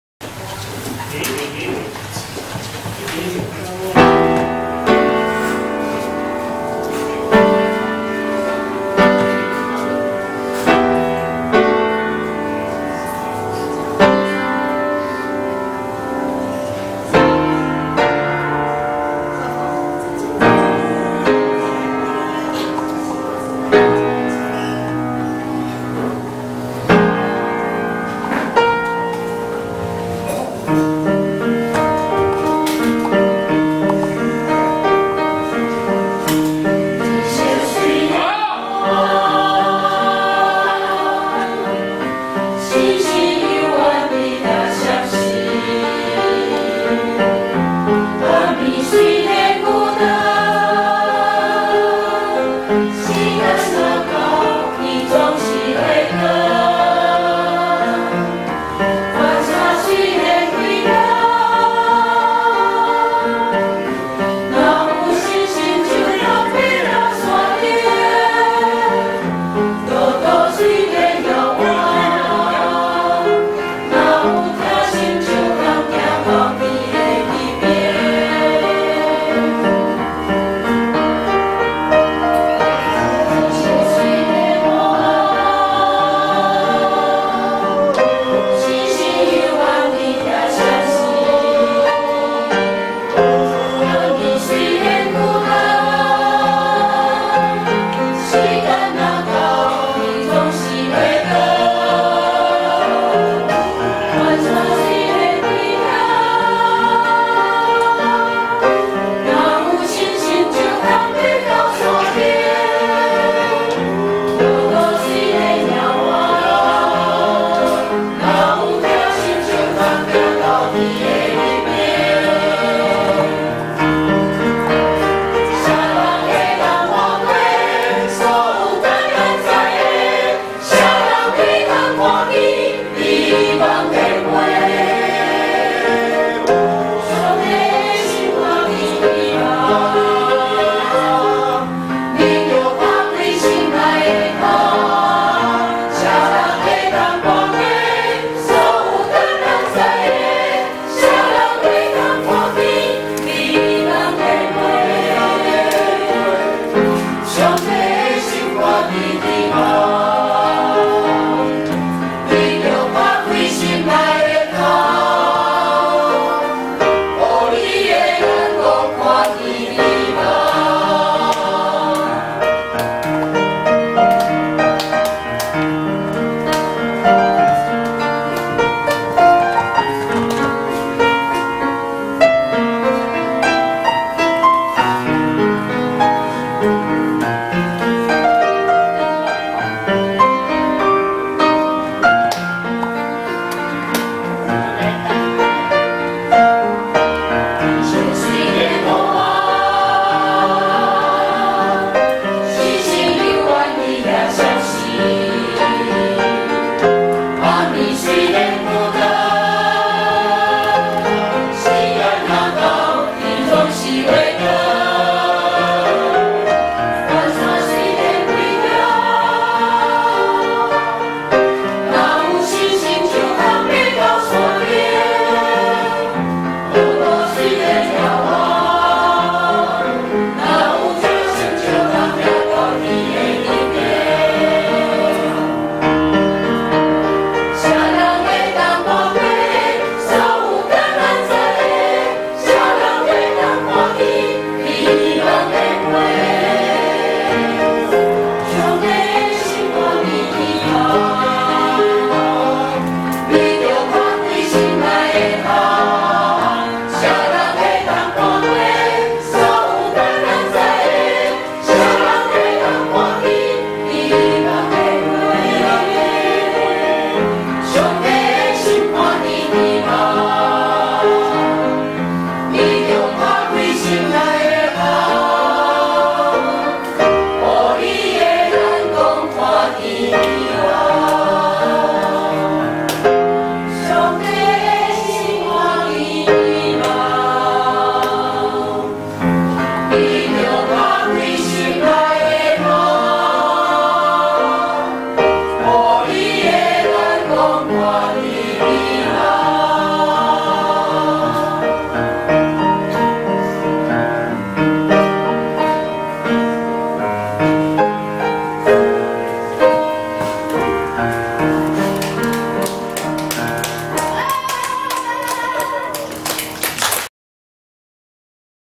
四結教會-竹林養護院-眼光&舞
四結教會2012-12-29於礁溪竹林養護院關懷演出-眼光&舞